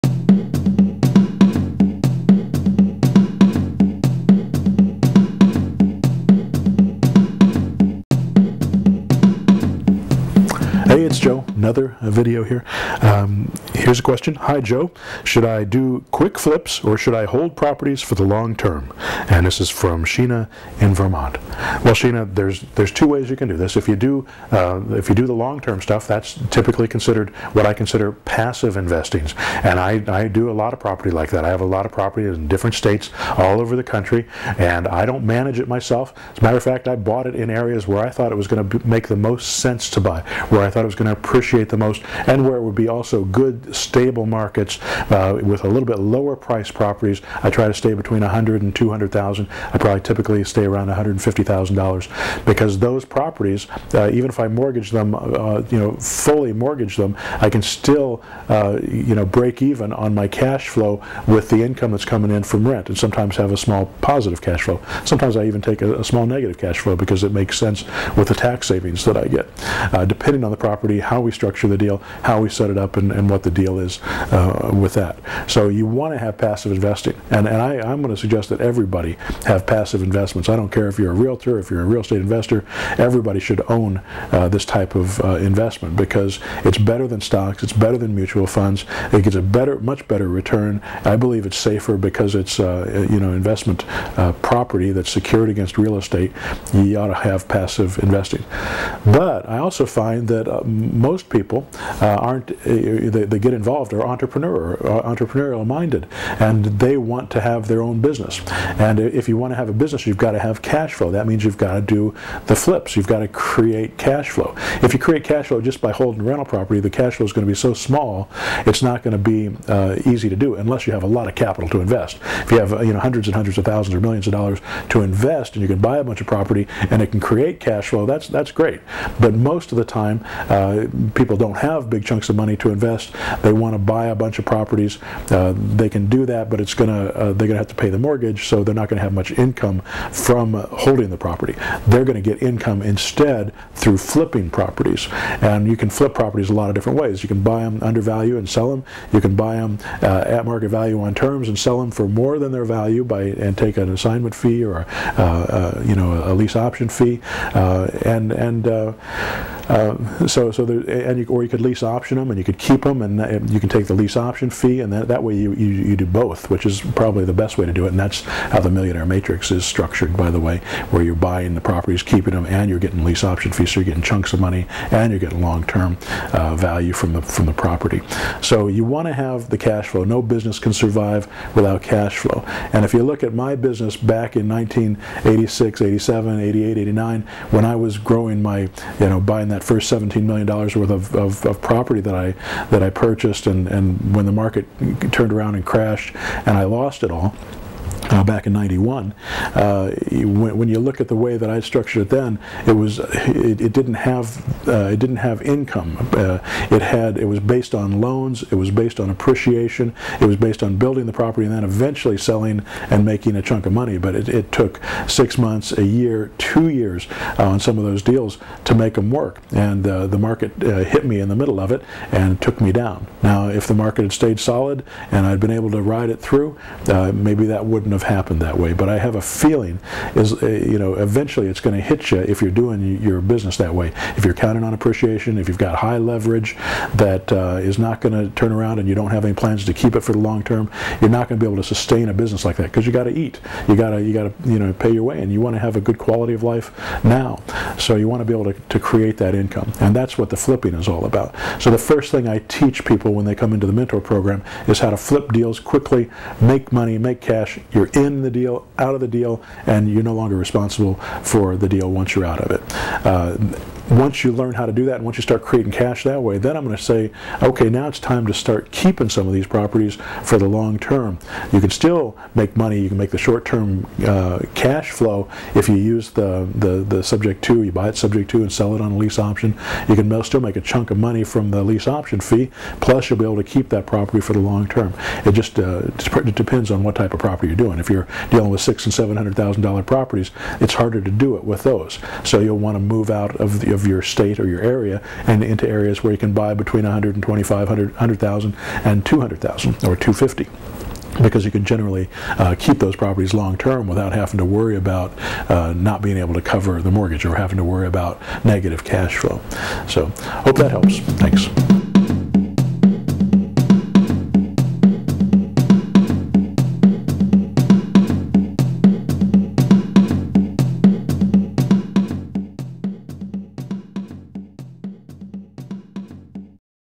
Realtors- The Secrets Of Building Wealth With Your Business - Real Estate Investing
How REALTORS can skyrocket their listing and sales commissions by using the marketing strategies of a master real estate investor. Get your free 65 minute recorded teleseminar

Read the Transcript:

I recommend everyone have passive investments, but to have a business, you need to have cash flow. Here's some of the advantages of passive investing and some tips on how to create cash flow and passively invest in real estate.

"Should I do quick flips or should I hold properties for the long term?" -- Sheena in Vermont

Joe: Sheena, there are two ways that you can do this. If you do them long term, that's typically what I would consider passive investing. I do a lot of property like that. I have a lot of property in different states all over the country. I don't manage it myself. As a matter of fact, I bought it in areas where I thought it was going to make the most sense to buy, where I thought it was going to appreciate the most and it would also be in good stable markets with a little bit lower priced properties.

Joe: I tried to stay between $100,000 and $200,000. I typically stay at around $150,000 because with those properties, even if I fully mortgage them, I can still break even on my cash flow with the income that's coming in from rent, and sometimes have a small positive cash flow. Sometimes I even take a small negative cash flow because it makes sense with the tax savings that I get, depending on the property and how we structure the deal and set it up.

Joe: So, you want to have passive investing. I'm going to suggest that everybody have passive investments. I don't care if you're a realtor or a real estate investor -- everybody should own this type of investment, because it's better than stocks, it's better than mutual funds and it gets a much better return. I believe it's safer because its investment property that's secured against real estate. You ought to have passive investing.

Joe: But, I also find that most people that get involved are entrepreneurial minded and they want to have their own business. If you want to have a business, you have to have cash flow. That means you have to do the flips; you have to create cash flow. If you create cash flow just by holding rental property, the cash flow's going to be so small that it's not going to be easy to do unless you have a lot of capital to invest.

Joe: If you have hundreds and hundreds of thousands or millions of dollars to invest and you can buy a bunch of property and it can create cash flow, that's great, but most of the time, people don't have big chunks of money to invest. They want to buy a bunch of properties, and they can do that, but they're going to have to pay the mortgage so they're not going to have much income from holding the property. They're going to get income instead through flipping properties.

Joe: You can flip properties a lot of different ways. You can buy them under value and sell them. Or, you can lease option them and keep them and take the lease option fee, and that way, you do both, which is probably the best way to do it.

Joe: That's how "The Millionaire Matrix" is structured, to where you're buying the properties, keeping them and getting the lease option fee, so that you're getting chunks of money and the long term value from the property.

Joe: No business can survive without cash flow. If you look at my business back in 1986/87/88/89, when I was buying that first 17 million dollars of property that I purchased and then the market turned around and crashed in 91' and I lost it all, with the way that I structured it then, it didn't have income.

Joe: It was based on loans. It was based on appreciation. It was based on building the property and then eventually selling and then making a chunk out of selling, but it took six months, a year or two years on some of those deals to make them work, and then the market hit me in the middle of it and it took me down. If the market had stayed solid and I'd been able to ride it through, maybe it wouldn't have happened that way.